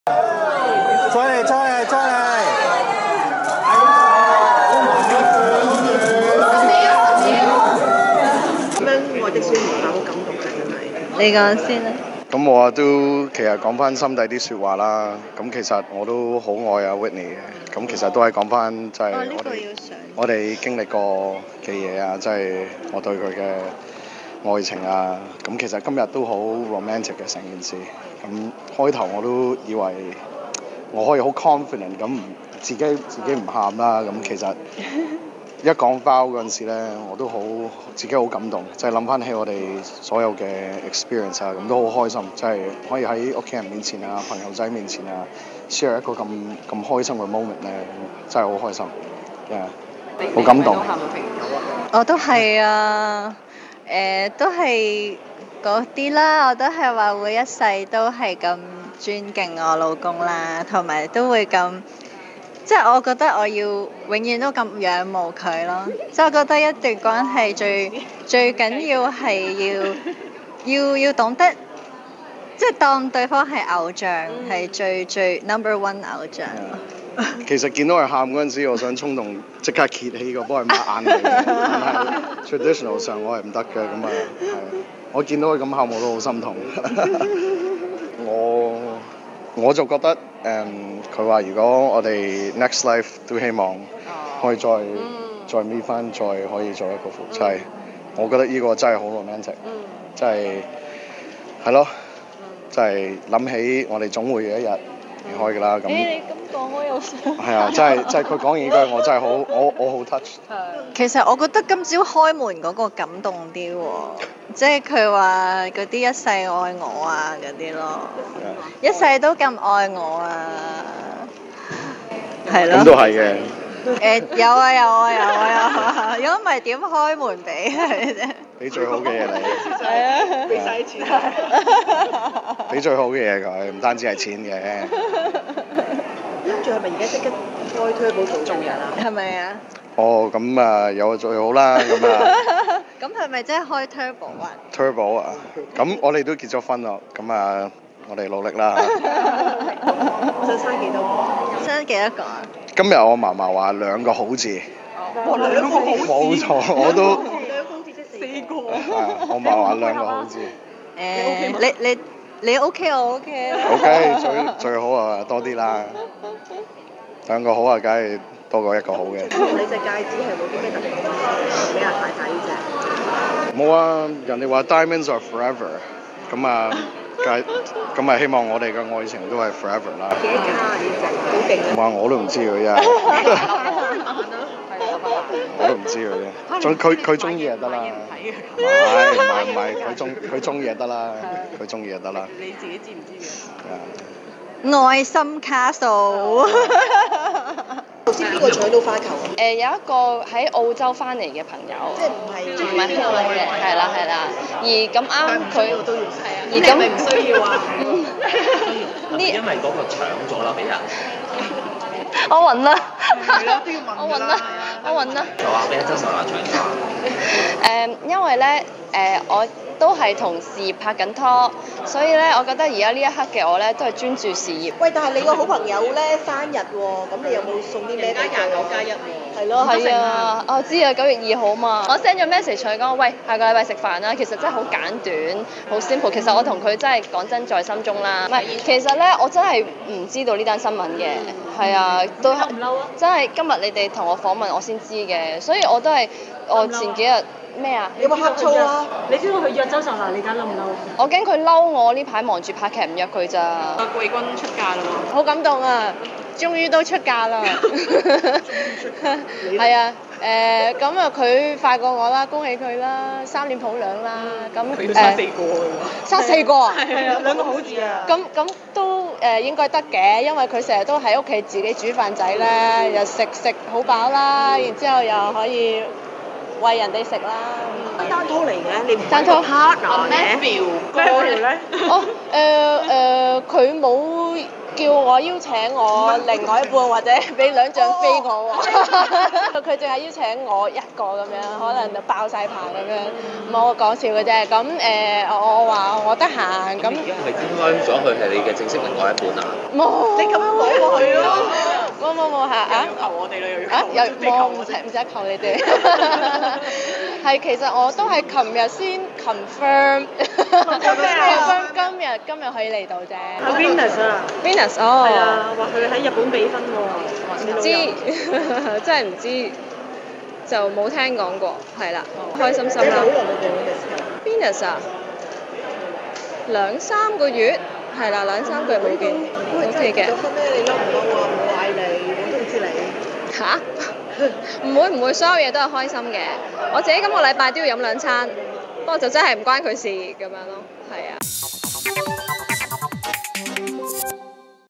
老公婚後想生兩個「好」字 許芷熒：你OK 我OK
2011年港姐季軍許芷熒（前名許亦妮），今日與拍拖兩年、從事印刷生意的男友Alex Fung結婚，在尖沙嘴筵開40多席婚宴。一對新人於傍晚會見傳媒，拍照時應要求咀嘴，場面相當溫馨。

許芷熒與老公Alex一同受訪，二人聲稱宣讀愛的宣言時，大家忍不住喊了出來。Alex說：「最感動是老婆說約定我下一世都要做夫妻，因為終有一日會離開，我想起這句好浪漫。」許芷熒指接新娘時，聽到老公講一世愛她最為感動，她更笑說：「仲聽到老公話畀晒啲錢我，否則不會開到門啦！」

Alex表明有共識婚後不設防，嫲嫲希望他們生兩個「好」字，許芷熒即回應老公：「生4個，你OK、我OK！我11月生日，到時會一次過度蜜月。」許芷熒深得老公疼惜，現場目測其鑽石婚戒約3、4卡，Alex笑說：「鑽石代表永恆的愛，她鍾意就得。」許芷熒透露婚戒是愛心卡數，身穿的性感婚紗也獲老公批准。